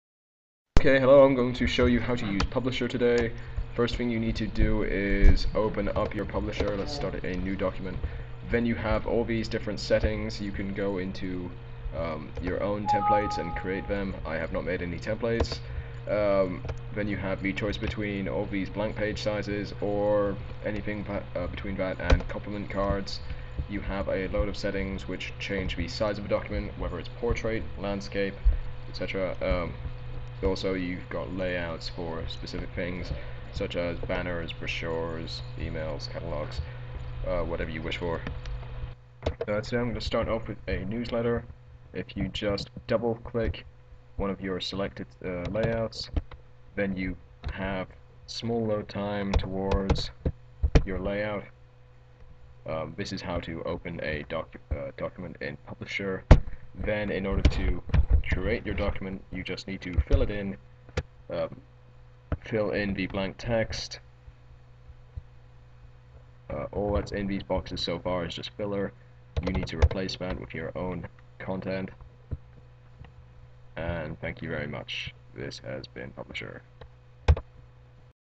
Microsoft Publisher for Beginners
Today I show you how to start Microsoft Publisher. This should allow you to confidently access Publisher, as well as select the layout of your documents.